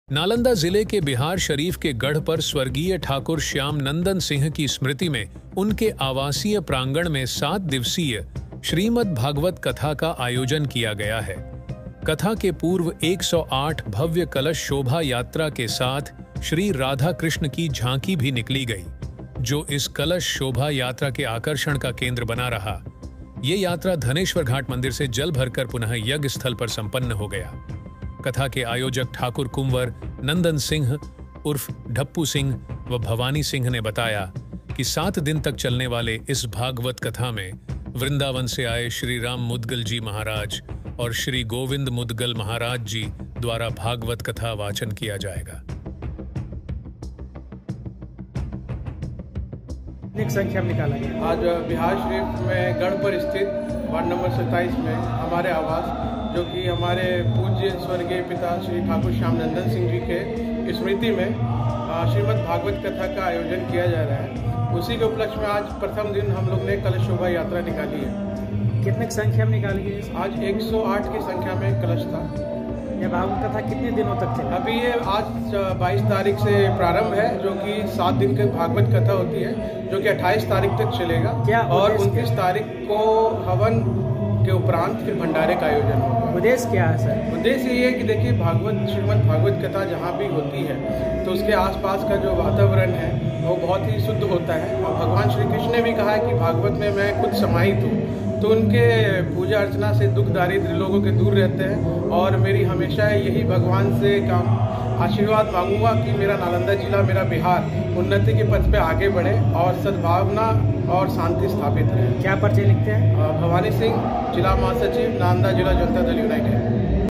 Biharsharif में भव्य कलश यात्रा के साथ आरंभ हुआ 7 दिवसीय श्रीमद भागवत कथा
7 day Shrimad Bhagwat Katha started with grand Kalash Yatra in Biharsharif